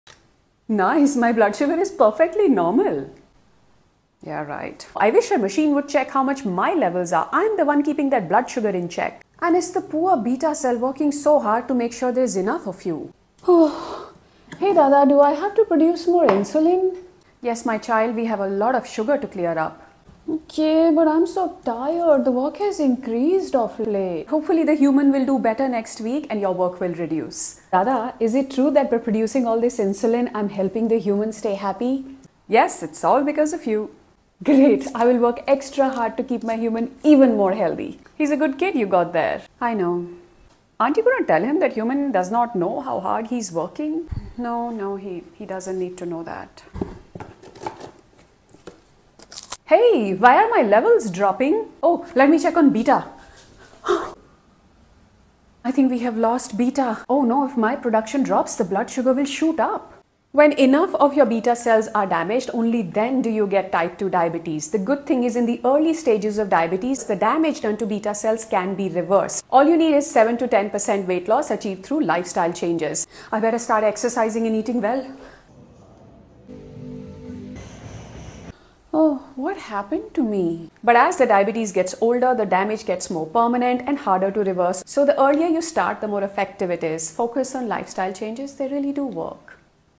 Diabetes & beta cells | Namita Piparaiya Skits
Type 2 diabetes has two quintessential characteristics
1. Insulin Resistance
2. Beta Cell Damage
Due to insulin resistance Beta cells have to produce more and more insulin, and at some point become dysfunctional before eventually becoming permanently damaged.
As per one study, people who had diabetes for more than 6 years were put on a weight-loss regimen. And all those who maintained their weight loss stayed in remission for the entire duration of the study (2 years). They also recovered most of their beta cell function as overall weight loss also removes excess fat from these cells.
But those who re-gained weight or gained more weight, failed to keep diabetes in remission. For Diabetes Prevention, 7% weight loss is recommended. However, in the above study those who achieved remission lost around 15% weight in 4 months and then maintained that weight loss for the remaining period.
💡Lifestyle changes that include diet, exercise and stress management are more effective than medication in preventing diabetes, so please give them your best shot. 💖